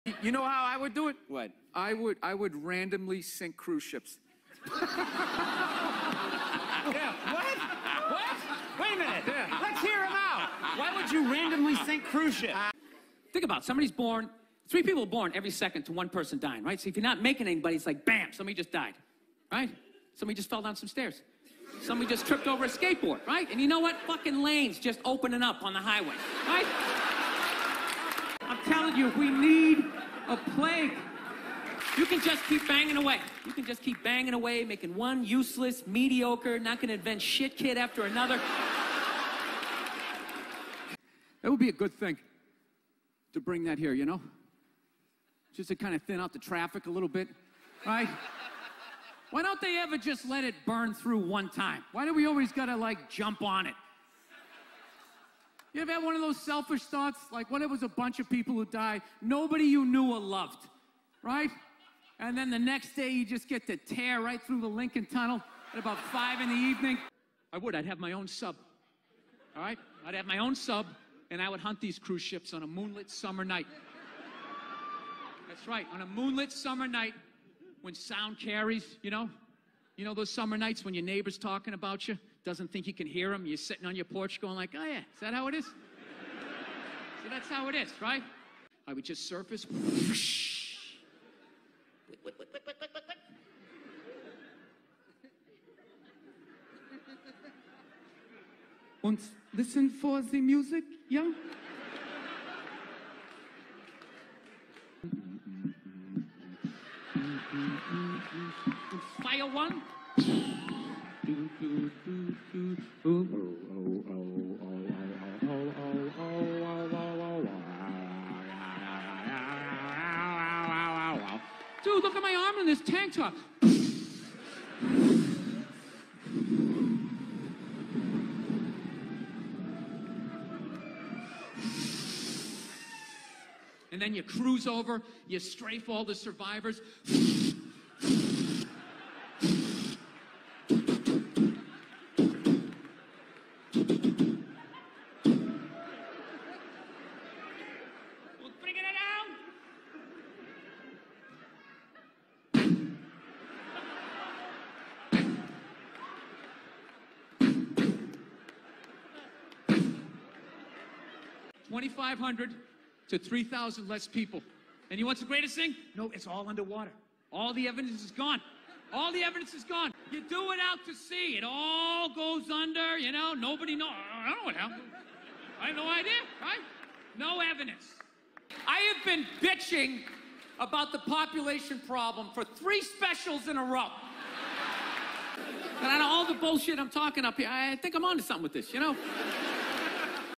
Bill Burr's Evolution of Population Control
Bill Burr shares his twistedly morbid ideas of shrinking the world population to ease the traffic at rush hour. From world pandemics, to strategically sinking cruise ships. Nothing is off limits with Burr's pursuit of thinning the herd.